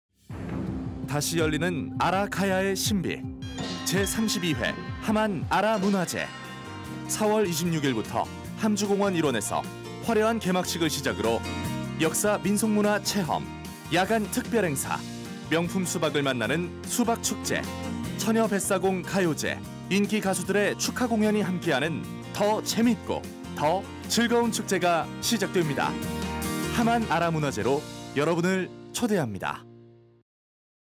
[제32회 함안아라문화제 SPOT] 함안으로의 초대! 다시 열리는 아라가야의 신비! (2019.04월)
The 32th Haman ARA Cultural Festival
“함안으로의 초대! 다시 열리는 아라가야의 신비! "

4월 26일부터 함주공원 일원에서 화려한 개막식을 시작으로
역사민속문화체험, 야간특별행사, 명품수박을 만나는 수박축제,
처녀뱃사공 가요제 인기가수들의 축하공연이 함께하는
더 재미있고 더 즐거운 축제가 시작됩니다.
함안 아라문화제로 여러분을 초대합니다.

☞ 일시 : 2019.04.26(금) ~ 04.28(일) / 3일간
☞ 장소 : 함안군 함주공원 및 공설운동장 일원
☞ 주요 행사
  - 아라가야 왕성 조성-불꽃정원 연출
  - 야간 특별 주제 공연 "아라가야의 혼! 아라가야 고취대"
  - 아라가야 역사문화체험
  - 아라가야 병영문화체험
  - 제12회 함안처녀뱃사공 전국가요제
    (청하, 박미경, 금잔디, 강진, 진시몬)
  - 제26회 함안수박축제
  - 백양 조정규 화백 유작전